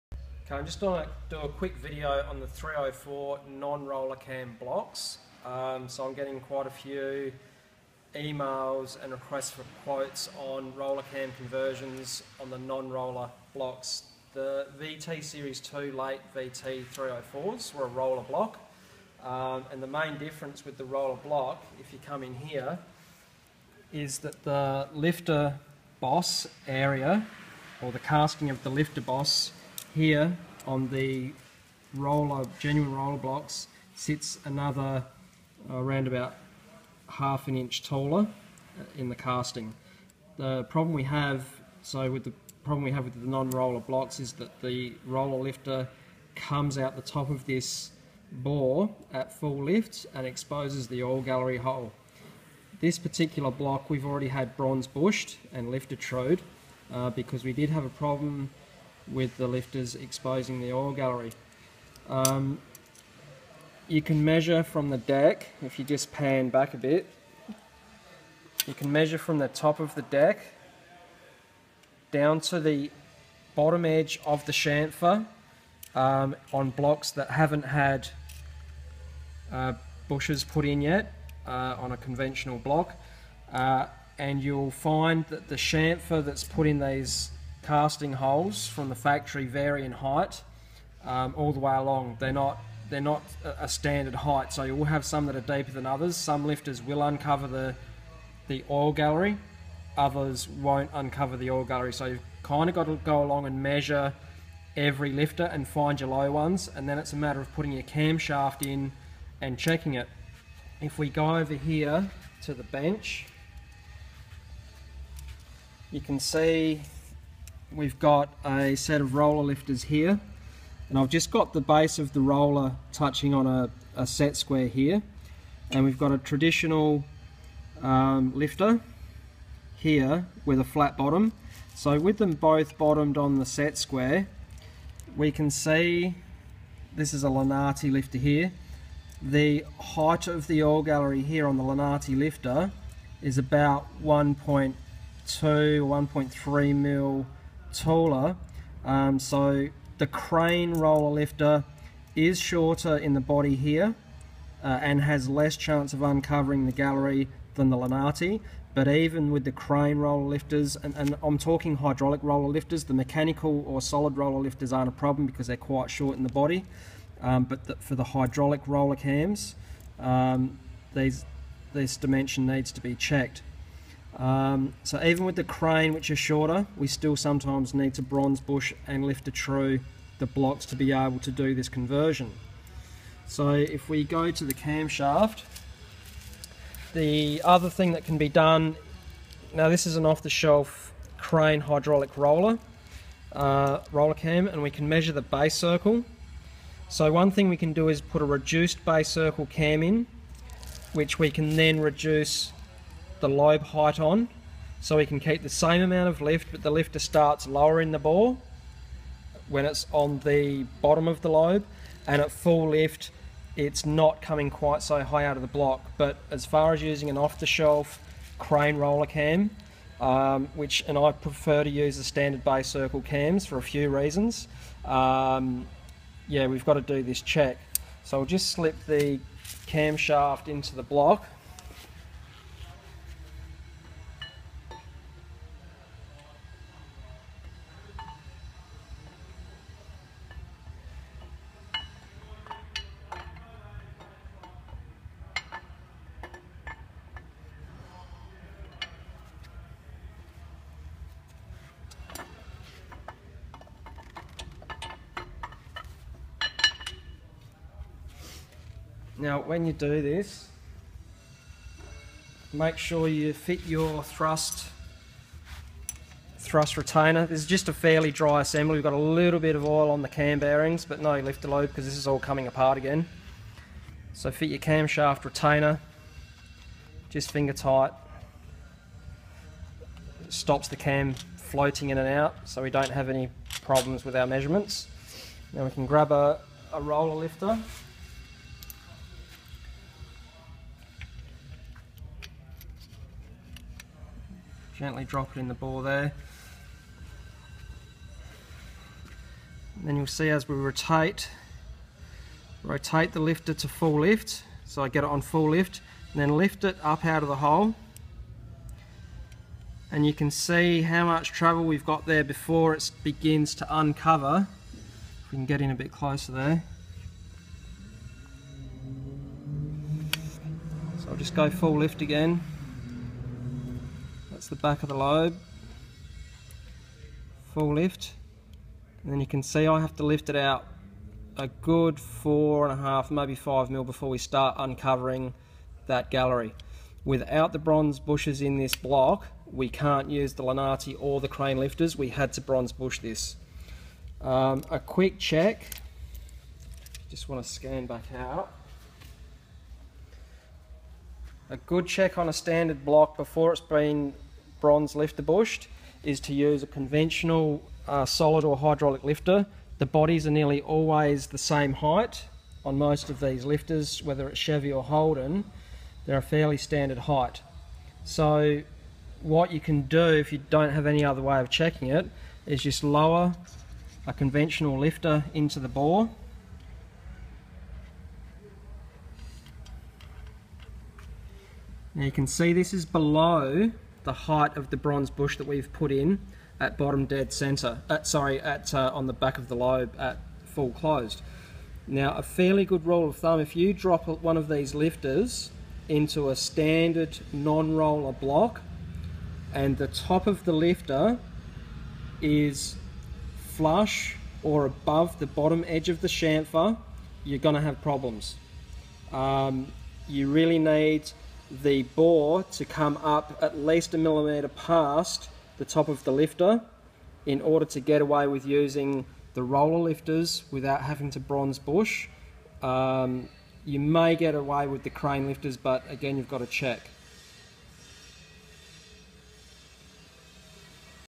Holden 304 hydraulic roller retro fit
Checking lifter bore heights and checking the large chamfer in the tops of the non roller 304 lifter bores. The crane hydraulic roller lifters are approximately 0.050" shorter than the lunati lifters, but on some blocks with deep chamfers, bronze bushing and lifter true may still be required for the conversion with off the shelf CRANE hydraulic roller cams.